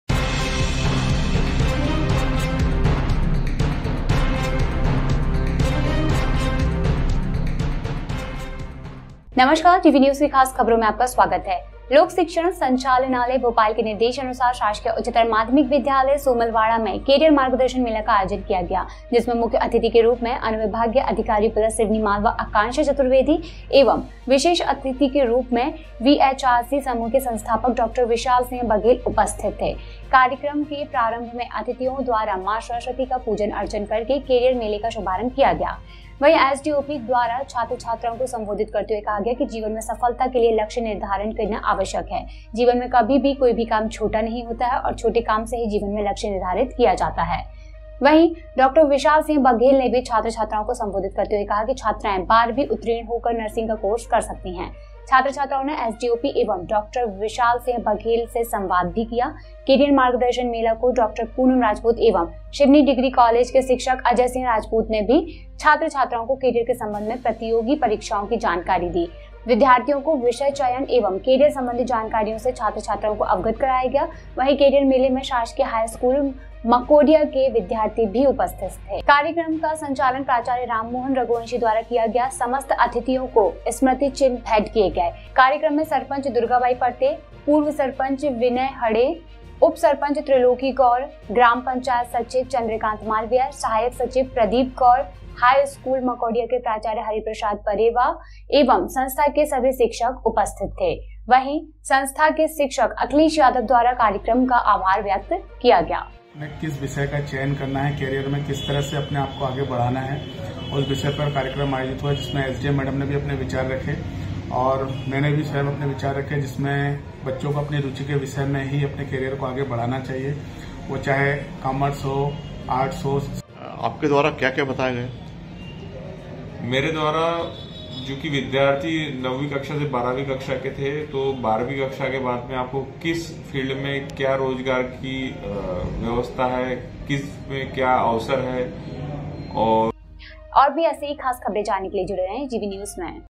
Seoni Malwa: Government Higher Secondary School Somalwada में Career मार्गदर्शन मेला का आयोजन किया
सिवनी मालवा लोक शिक्षण संचालनालय भोपाल के निर्देशानुसार शासकीय उच्चतर माध्यमिक विद्यालय सोमलवाड़ा में केरियर मार्गदर्शन मेला का आयोजन किया गया जिसमें मुख्य अतिथि के रूप में अनुविभागीय अधिकारी पुलिस सिवनी मालवा आकांक्षा चतुर्वेदी एवं विशेष अतिथि के रूप में बीएचआरसी समूह के संस्थापक डॉ विशाल सिंह बघेल उपस्थित थे कार्यक्रम के प्रारंभ में अतिथियों द्वारा मां सरस्वती का पूजन अर्चन करके केरियर मेले का शुभारंभ किया गया एसडीओपी द्वारा छात्र छात्राओं को संबोधित करते हुए कहा कि जीवन में सफलता के लिए लक्ष्य निर्धारण करना आवश्यक है जीवन में कभी भी कोई काम छोटा नहीं होता छोटे से काम से ही जीवन में लक्ष्य निर्धारित किया जाता है

As per the instructions of Seoni Malwa Directorate of Public Education, Bhopal, a career guidance fair was organized at Government Higher Secondary School, Somalwada, in which Sub-Divisional Officer Police Seoni Malwa, Akanksha Chaturvedi, and Dr. Vishal Singh Baghel, founder of BHRC Group, were present as the chief guest. At the beginning of the program, the career fair was inaugurated by worshiping Maa Saraswati by the guests. Addressing the students, the SDOP said that it is necessary to set goals for success in life, no work is ever small in life, even small tasks goal in life is determined by